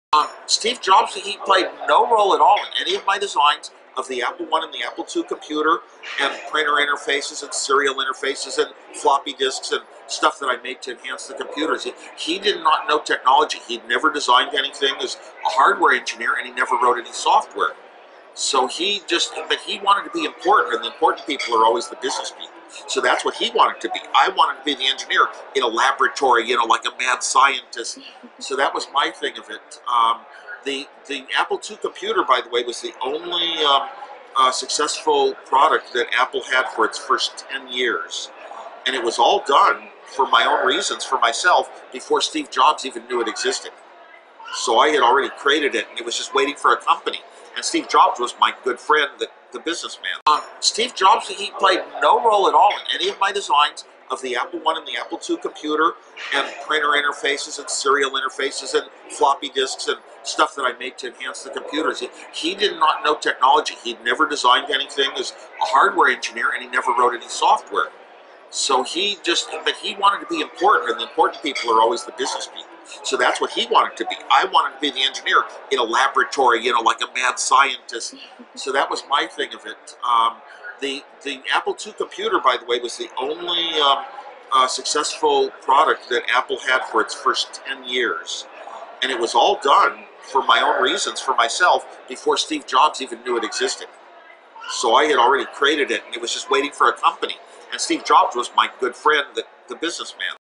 Jobs ‘played no role at all’ in design of Apple’s first computers: Wozniak
September 03, 2015: Steve Wozniak has said that Steve Jobs had "no role" in design of the original Apple computers. (YT: Reach A Student)
ftecA

In what will likely come as little surprise for many engineers and Apple aficionados, tech genius Steve Wozniak claims business partner Steve Jobs had no hand in the design of the first commercially successful Apple computers.

Mr Wozniak has confirmed as much in an interview with Sarina Khemchandani, the teenage founder of ReachAStudent, a website that helps students communicate with potential mentors.

During the interview, Ms Khemchandani asked Mr Wozniak what role Steve Jobs played during the creation of the original Apple I and Apple II computers.